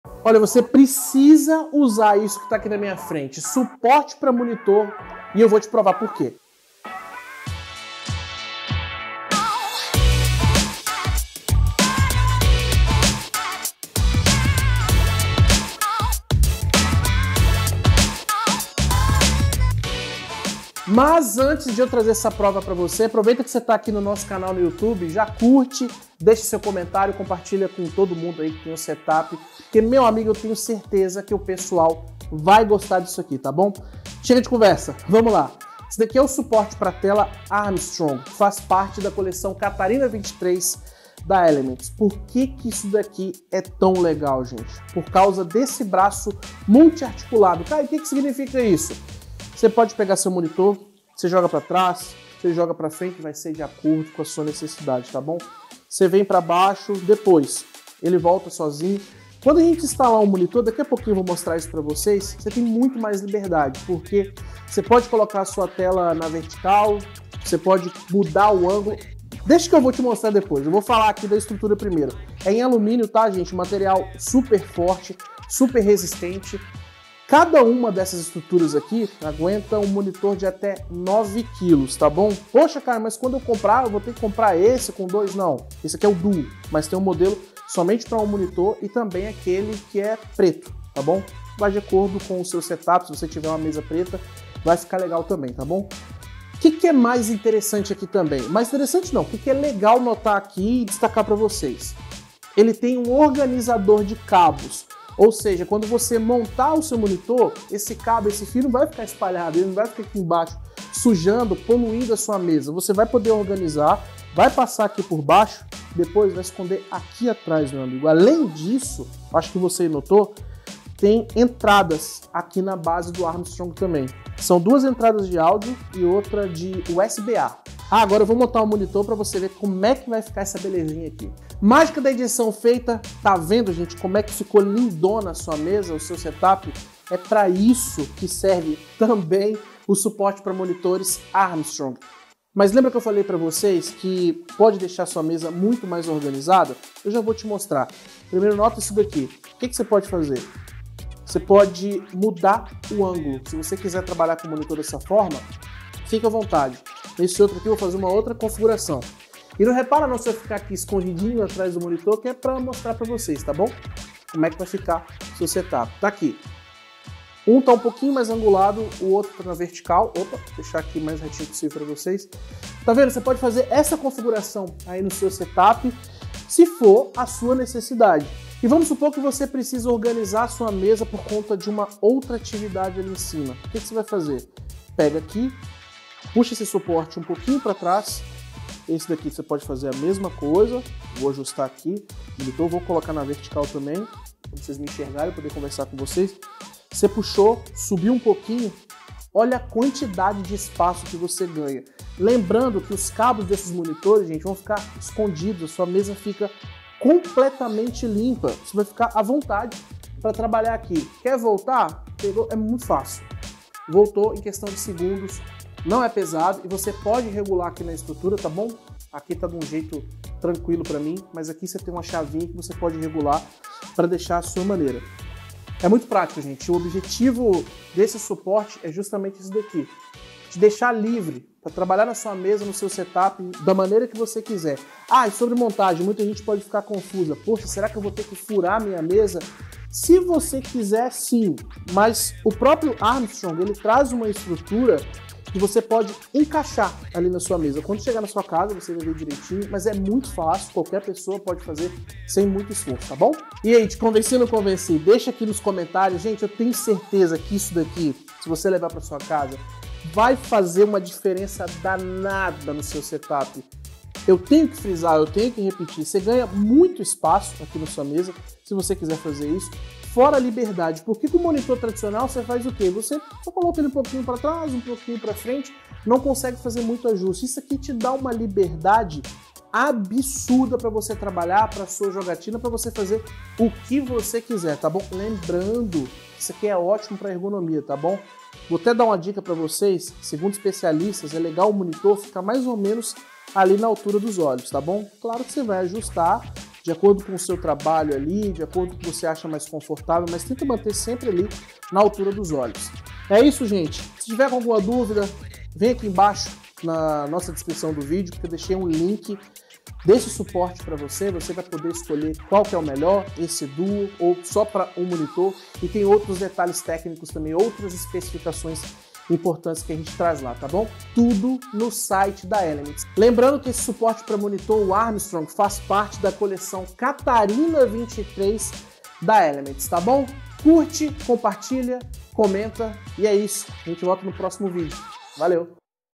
A peça que faltava para O SEU SETUP: SUPORTE ARTICULADO PARA MONITOR
O desafio de quem precisa ficar horas na frente do computador: manter a mesa o mais organizada possível.

Dá para fazer isso, e anda a ter outros resultados muito interessantes, com o suporte para monitor Armstrong, da Elements.

Além de deixar a sua mesa livre, esse tipo de produto vai te dar liberdade para colocar o seu monitor do jeito que você precisa: angulado, na vertical, mais para cima, mais para baixo…

E tudo isso com alguns segundos, sem se esforçar ou ficar apertando ou afrouxando parafusos. No vídeo, apresentamos o Armstrong Duo, que serve para dois monitores. Mas a Elements também tem o modelo para um único monitor

Para facilitar a sua vida, o suporte também traz entradas USB, organizadores de cabo e muito mais. Assista o vídeo para entender porque essa é a peça que falta para o seu setup!

00:00 Abertura
00:35 Apresentado o suporte para monitor Armstrong, da Elements
01:12 Estrutura do suporte
02:11 Ajustes possíveis com o suporte articulado Armstrong
03:56 Montagem do suporte articulado
04:14 Encerramento